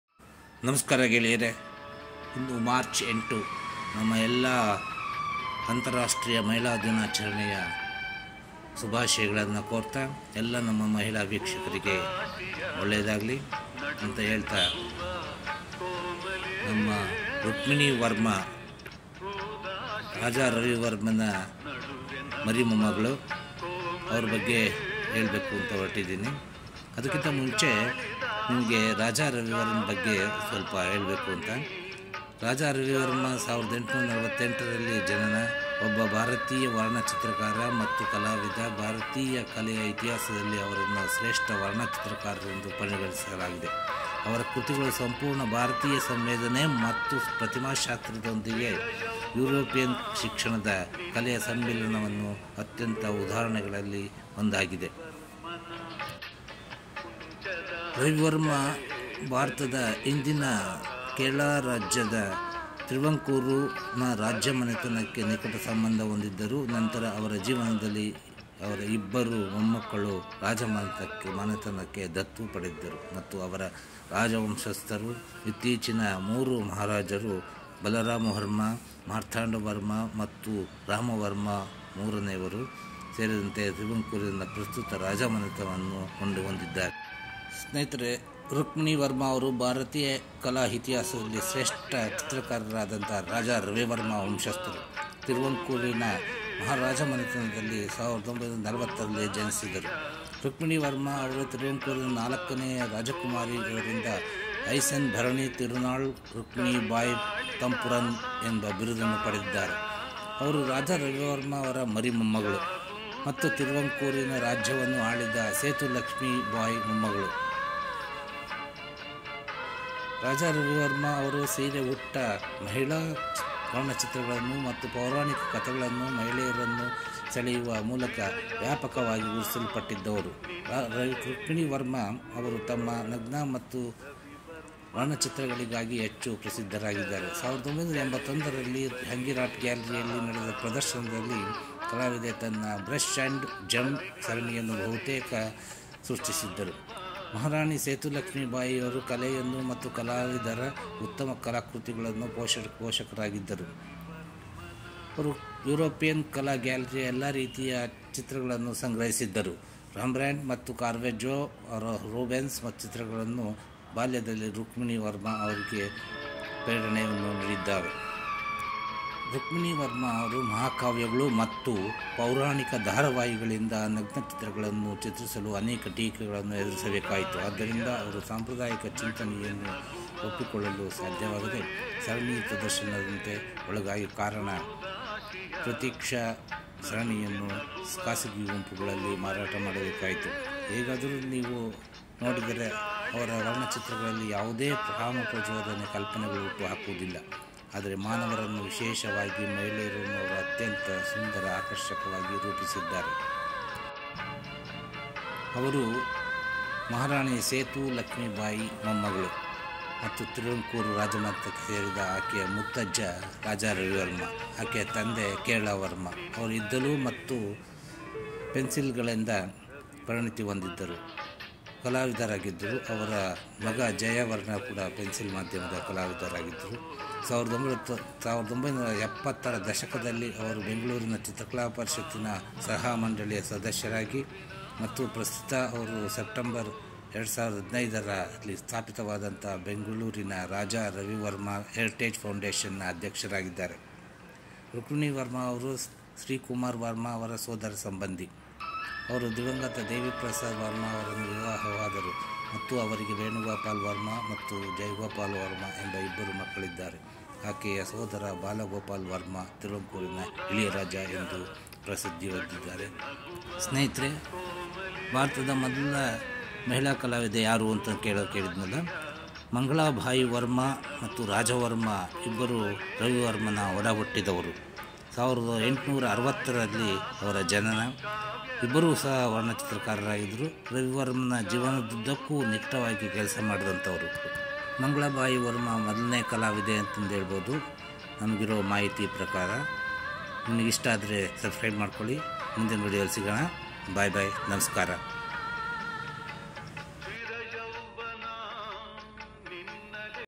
ಭಾರತದ ಮೋದಲ ಮಹಿಳಾ ಕಲಾವಿದೆ ಯಾರು ಗೋತ್ತೆ Raja Ravi Varma royal family
ಭಾರತದ ಮೋದಲ ಮಹಿಳಾ ಕಲಾವಿದೆ ಯಾರು ಗೋತ್ತೆ  Raja Ravi Varma royal family thiruvangoor

Rukmini Varma is the descendant of Raja Ravi Varma, the greatest painters in the Indian Art history. Born in 1940 in the royal family of Travancore, Rukmini Varma is the 4th Princess of Travancore titled Her Highness Bharani Tirunal Rukmini Bayi Tampuran. She is the great granddaughter of Raja Ravi Verma and granddaughter of Setu Lakshmi Bayi who ruled the state of Travancore.

While, Raja Ravi Varma was widely recognized for his saree clad women paintings and drawing women from the mythological stories, Rukmini Varma is highly acknowledged for her nudes and larger than life paintings. A self-taught artist her “Flesh and Gem” series in an exhibition at Jehangir Arts Gallery in 1981 almost created a stampede. Since Maharani Setu Lakshmi Bayi was a great patron of arts and artist, collecting all sort of paintings both from local artist and European galleries, the images painted by Rembrandt, Caravaggio and Rubens always fascinated Rukmini as a kid.

She began experimenting at an early age and this made her earn a full set of brushes as a gift from her uncle. Rukmini Varma faced many criticisms for painting nudes from epics and mythological series, she therefore had to sell the entire Pratiksha series in private groups because she was being objected from exhibiting the series to avoid provoking the orthodox thinking. However, her paintings if you see is not to evoke any erotic fantasies but to celebrate humans, especially women in their most graceful form.
Benakams